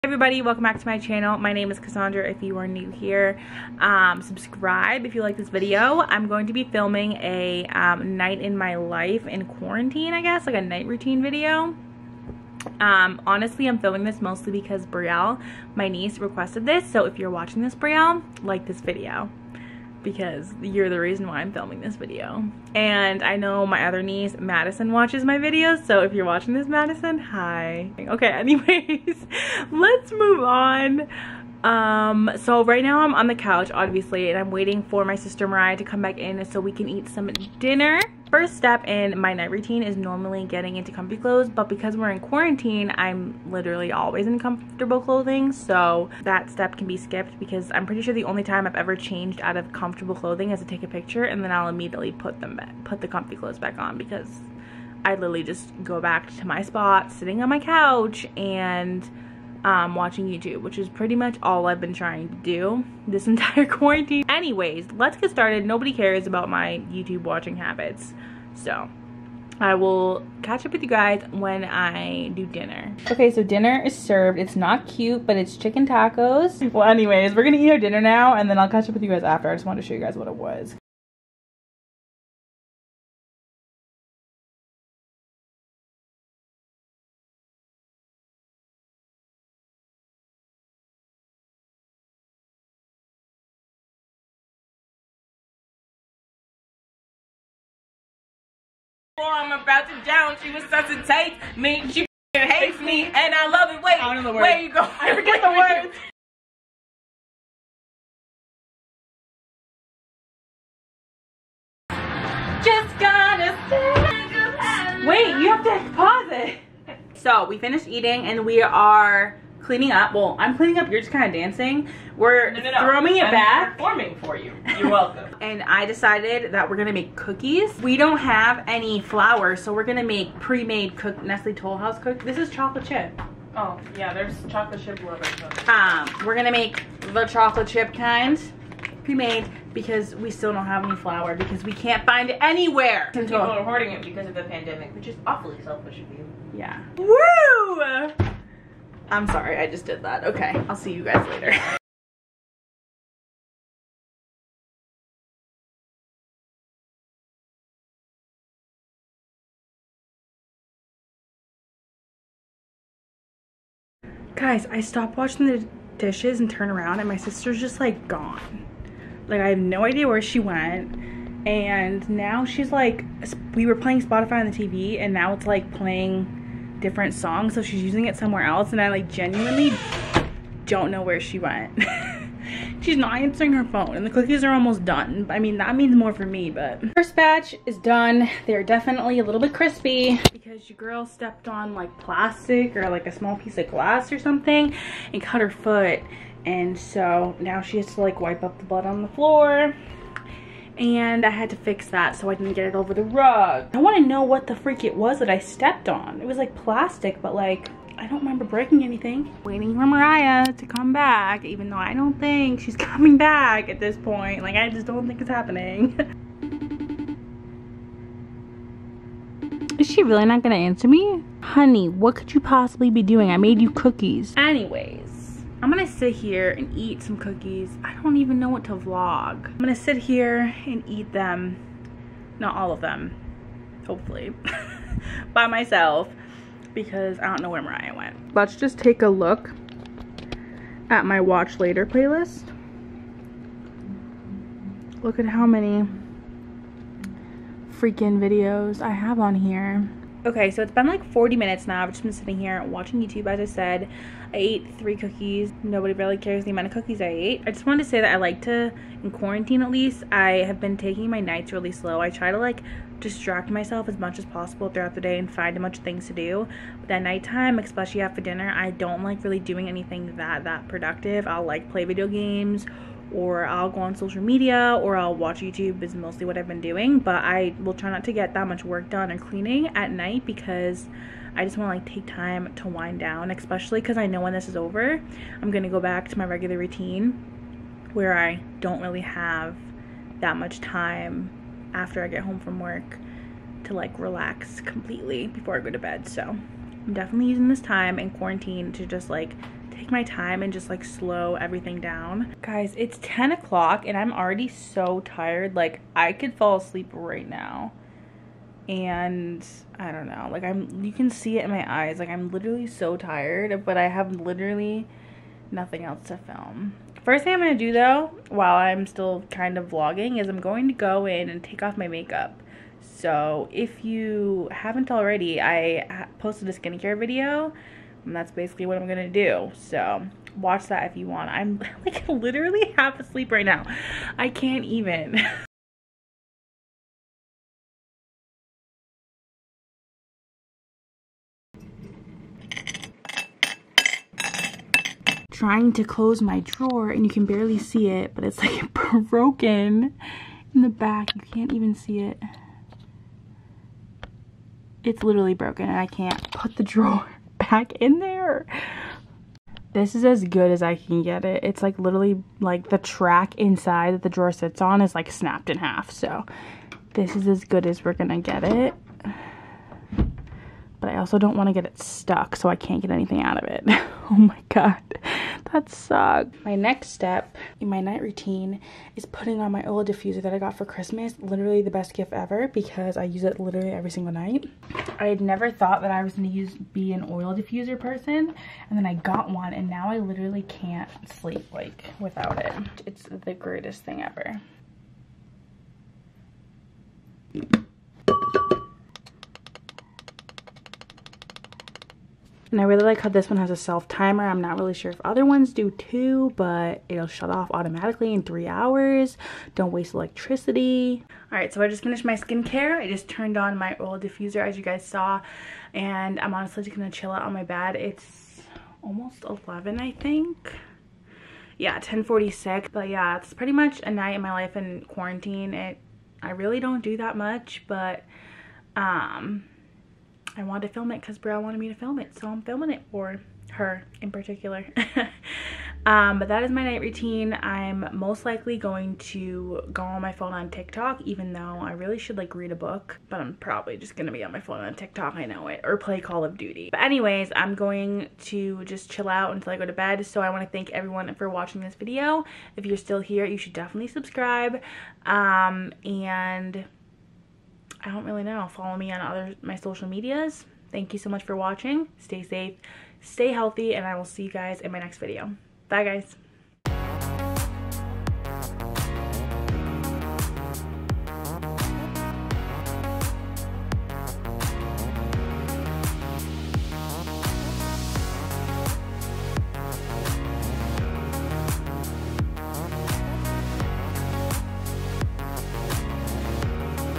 My Night in Quarantine
Hey Guys! Today's video is my night routine in quarantine!

Equipment
My Camera
My Lights